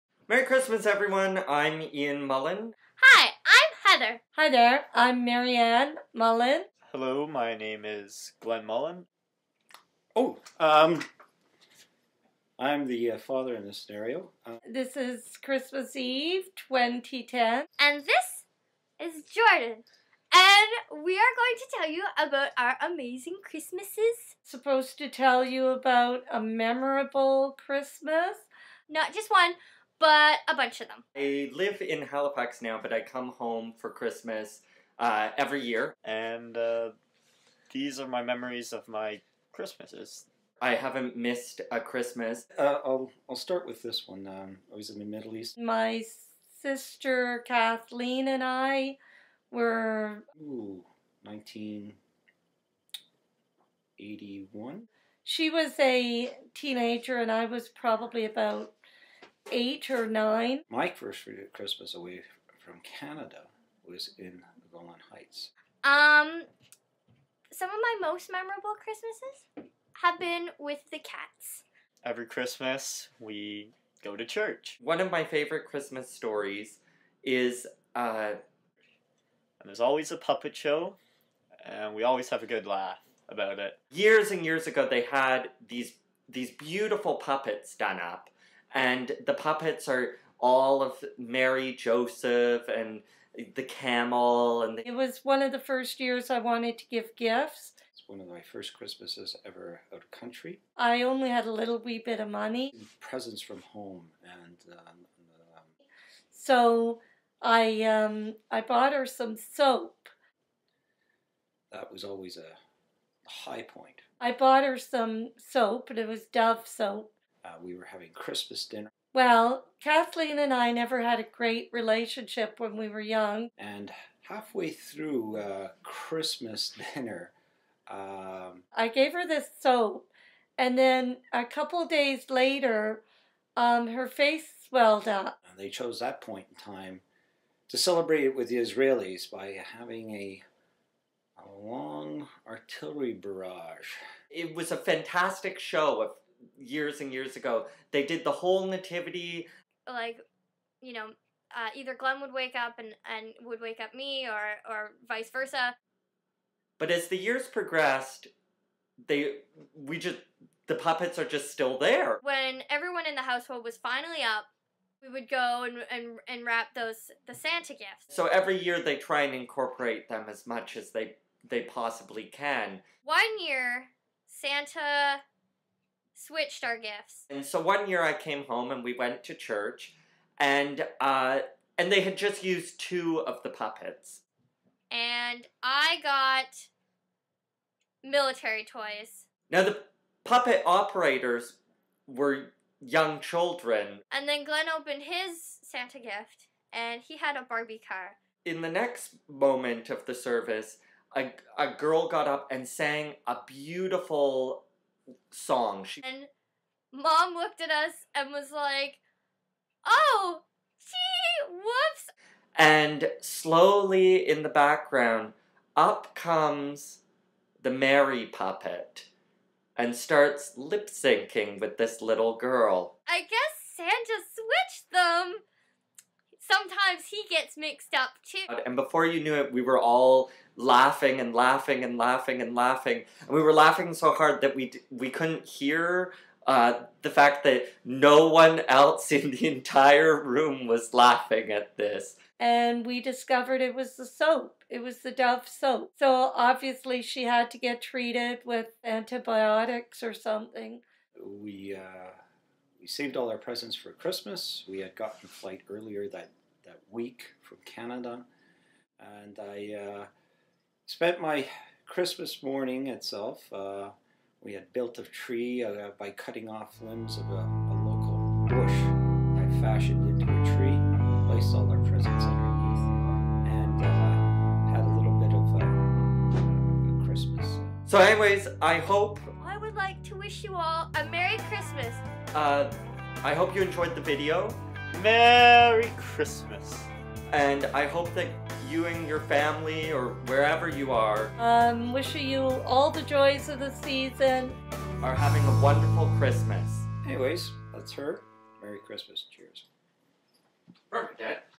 Christmas 2010: Best wishes from the Mullans for 2011!
Here are a few family stories about holidays past.  All the best to you and your family for 2011.

Starring:  The Mullan Family!
Ian Mullan
Marianne Mullan
Robert Mullan
Heather Mullan
Glen Mullan

*Note: There are a few rookie mistakes (such as the video being mirrored) but thankfully I'm a rookie.